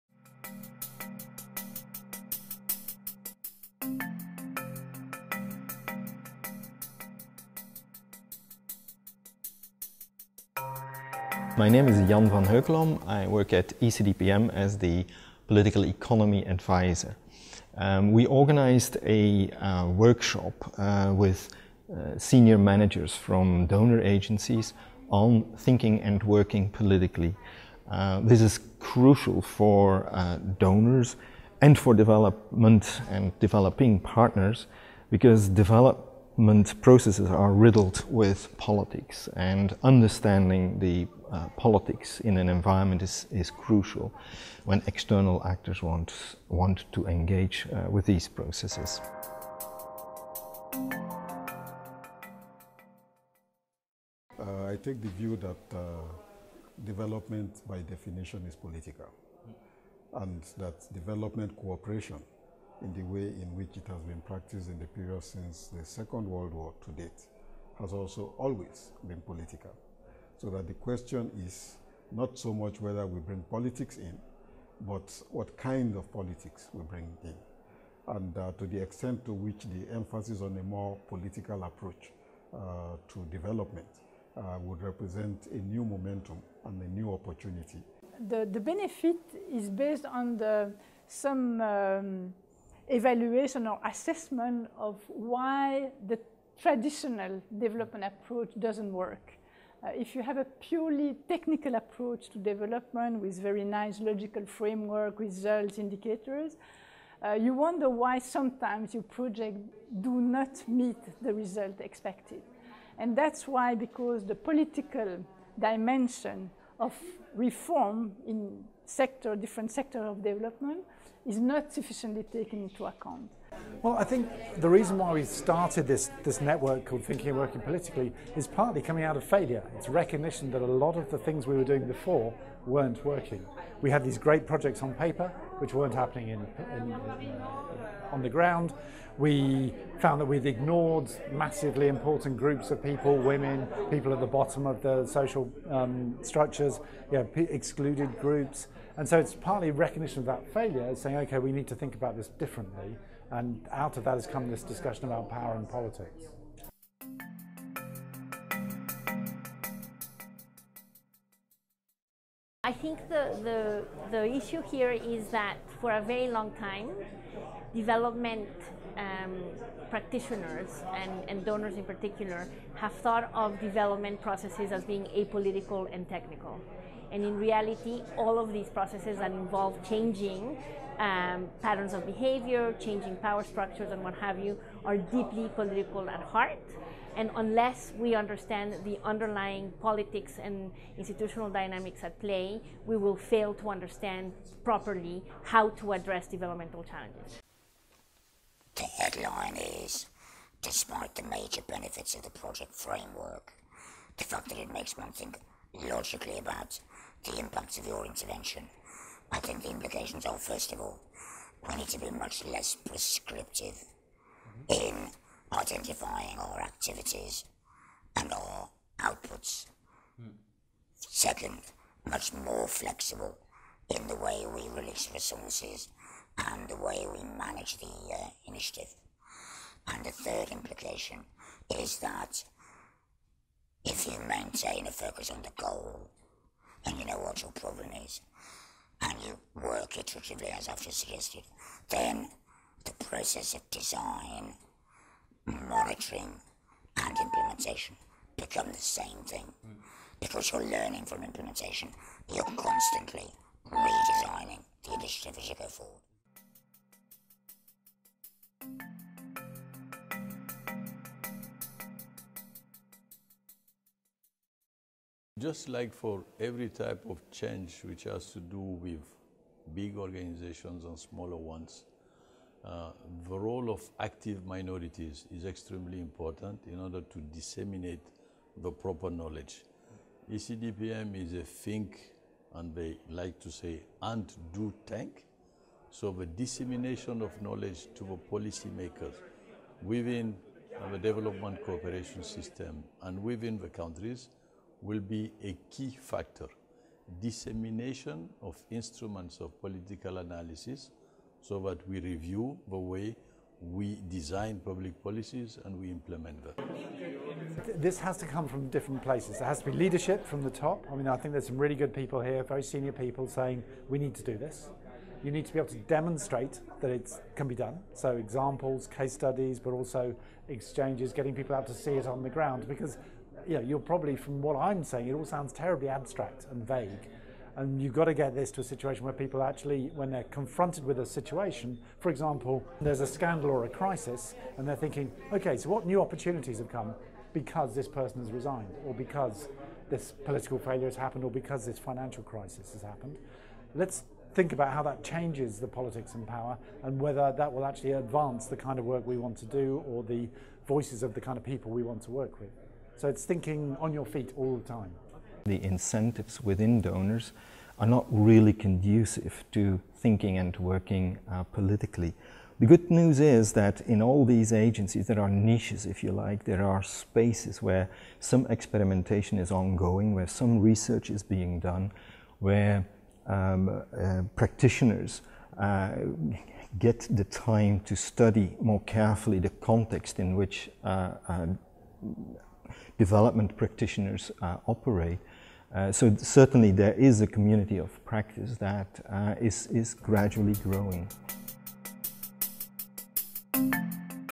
Development and politics, a new opportunity
On 8 May 2015, the European Centre for Development Policy Management (ECDPM) hosted its third annual meeting of the ECDPM Board of Governors, the Management and the institutional partners of the Centre. The topic of this year’s meeting was “Thinking and Working Politically in Development Post 2015”.

The aim of the meeting was to identify the benefits and risks of more politically informed approaches to development and to look into the practical implications of thinking and working politically (TWP) for development agencies and practitioners.